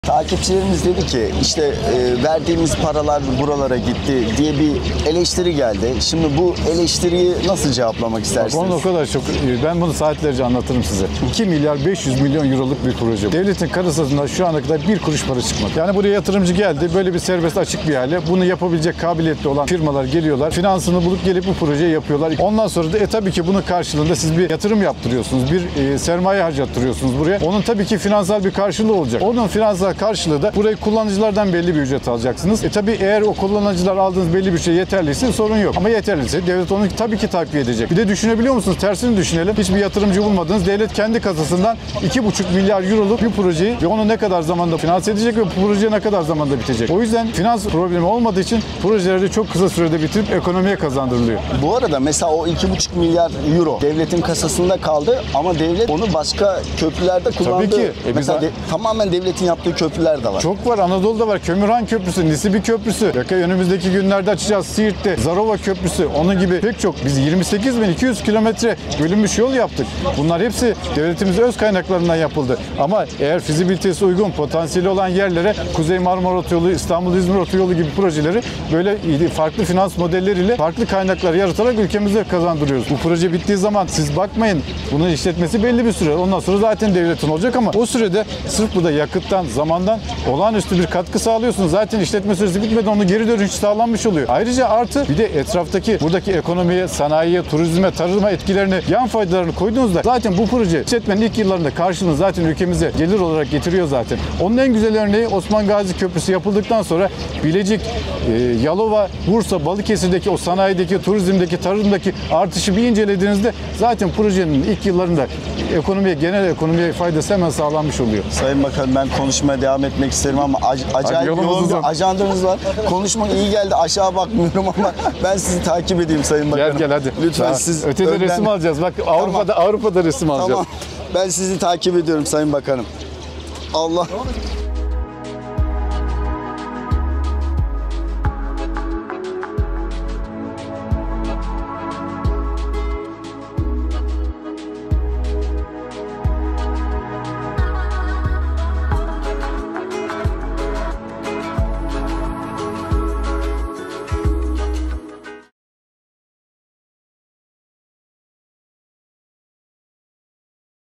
“Geçmediğimiz köprülere para mı ödüyoruz?” Merak edenler için yanıtladık.
“Geçmediğimiz köprülere para mı ödüyoruz?”

Merak edenler için yanıtladık.

📍1915ÇanakkaleKöprüsü, Asya Kule, 318 m